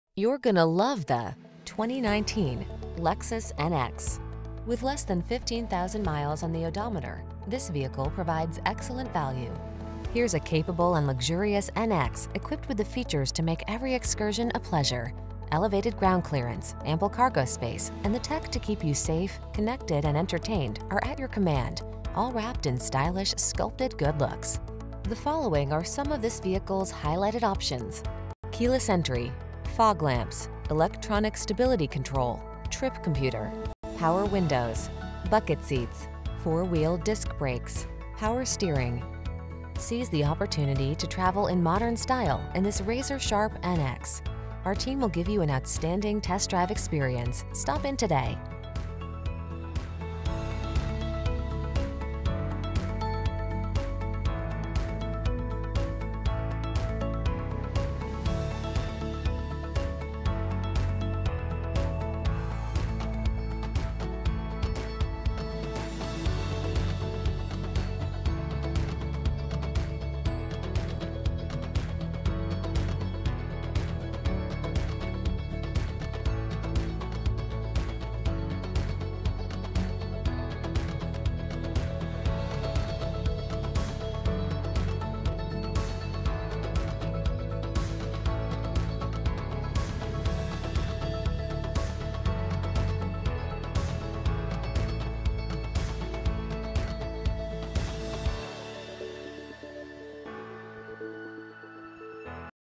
2019 Lexus NX North Miami, Fort Lauderdale, Hollywood, Aventura, North Miami Beach, FL C1710800
Silver Lining Metallic Used 2019 Lexus NX available in North Miami, Florida at Lexus of North Miami. Servicing the Fort Lauderdale, Hollywood, Aventura, North Miami Beach, FL area.

Recent Arrival! CARFAX One-Owner. Clean CARFAX. Odometer is 16395 miles below market average! Silver 2019 Lexus NX FWD 6-Speed Automatic 2.0L 16V DOHCbrbrbr22/28 City/Highway MPGbrbrAwards:br  * 2019 KBB.com 5-Year Cost to Own Awards   * 2019 KBB.com Brand Image Awards